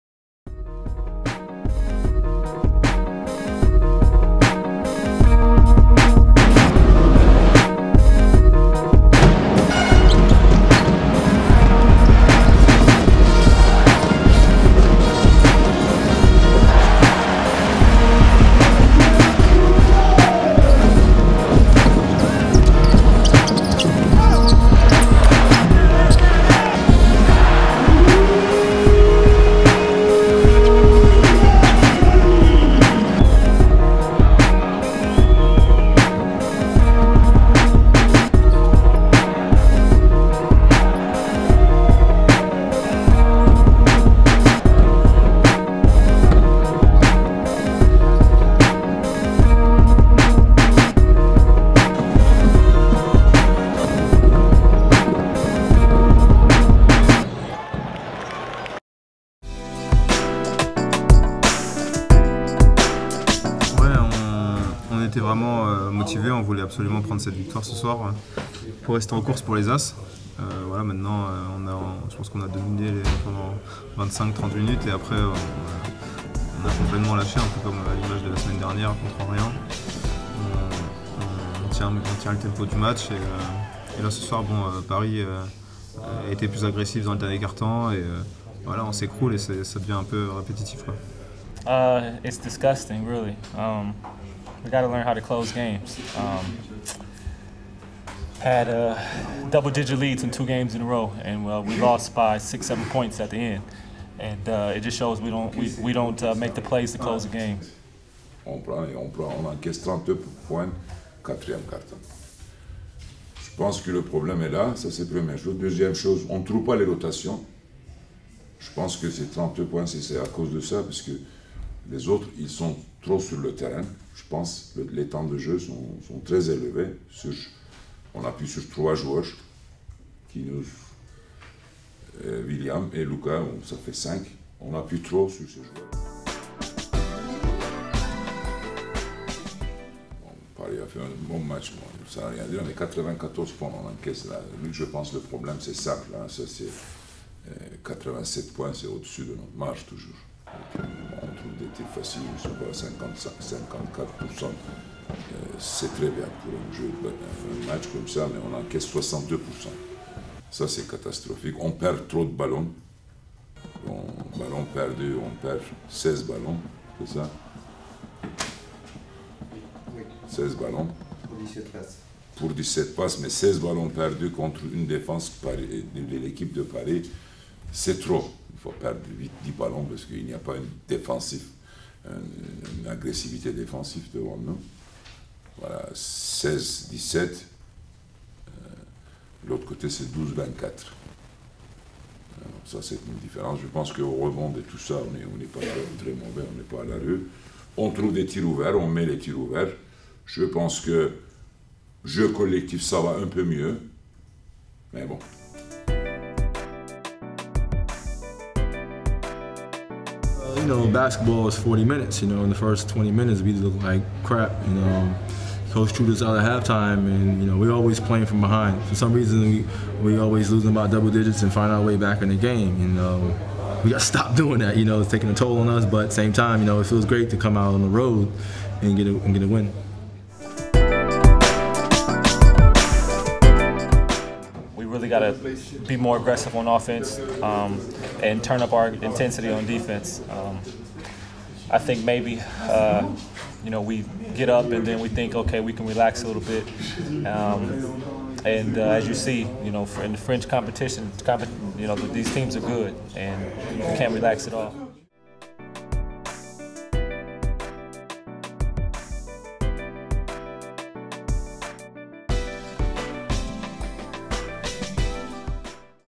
Highlights et Reactions Cholet 87-94 Paris Levallois
Extraits du match et réactions des principaux acteurs de cette rencontre du 9 décembre 2011 à la Meilleraie, Cholet.